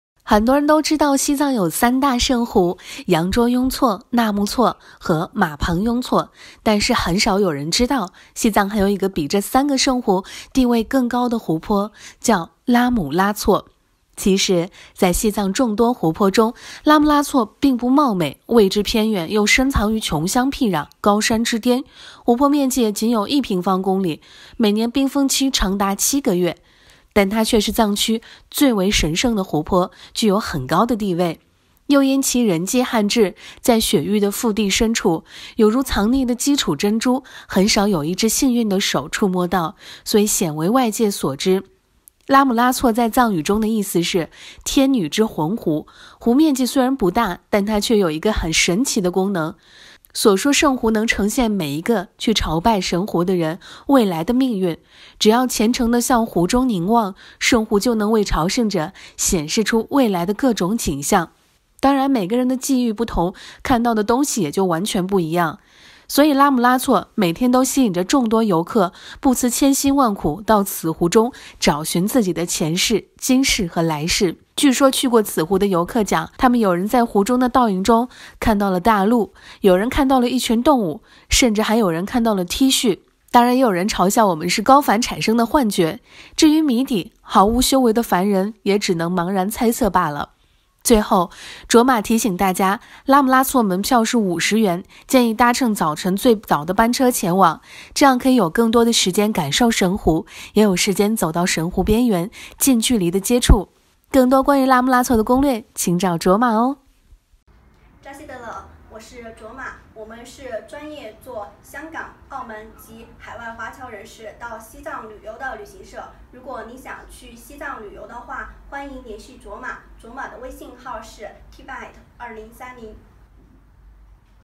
揭秘西藏最传奇的湖泊 - 拉姆拉措 - 可以看到自己的前世今生！
很多人都知道西藏有三大圣湖：羊卓雍错、纳木措和玛旁雍错，但是很少有知道，西藏还有一个比这三个圣湖地位更高的湖泊叫拉姆拉措。